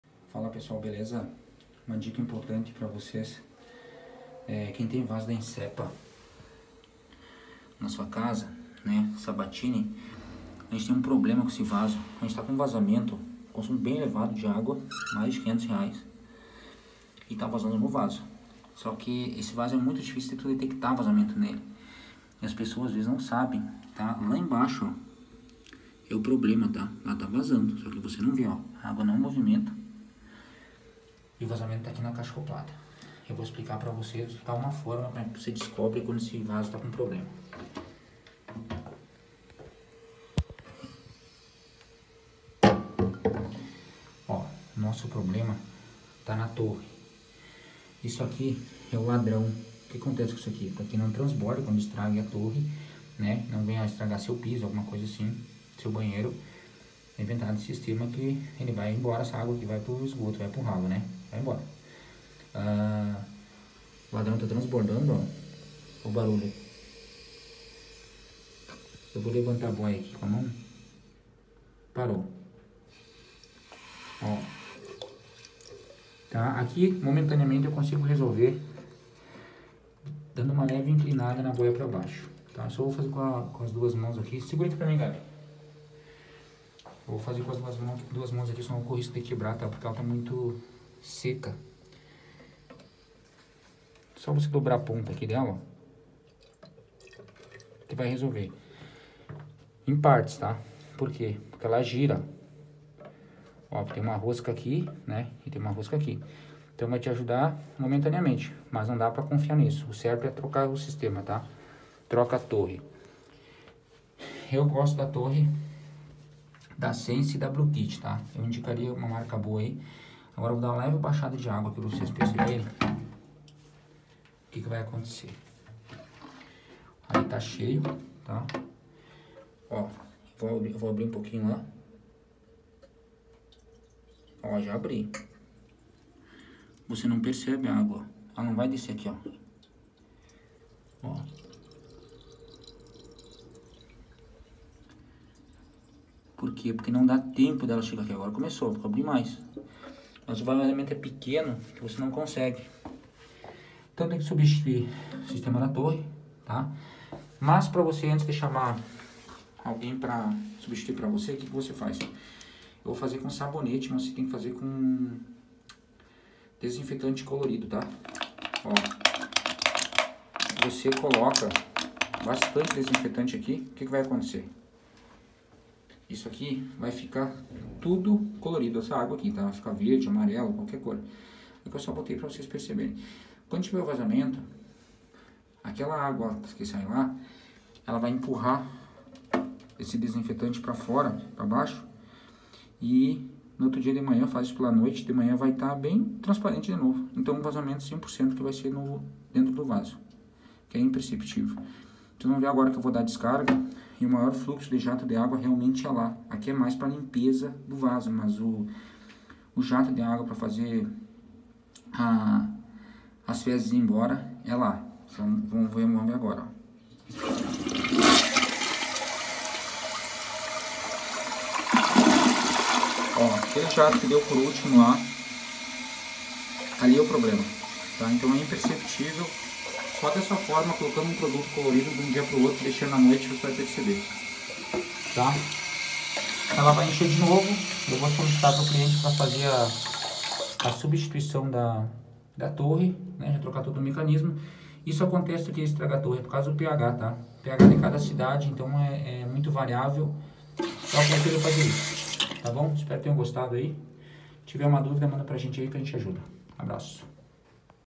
Vazamento de água no vaso sanitário.
Dica fácil de como identificar se o vaso sanitário está com vasamento.
 Faça o teste vc mesmo antes de chamar um profissional.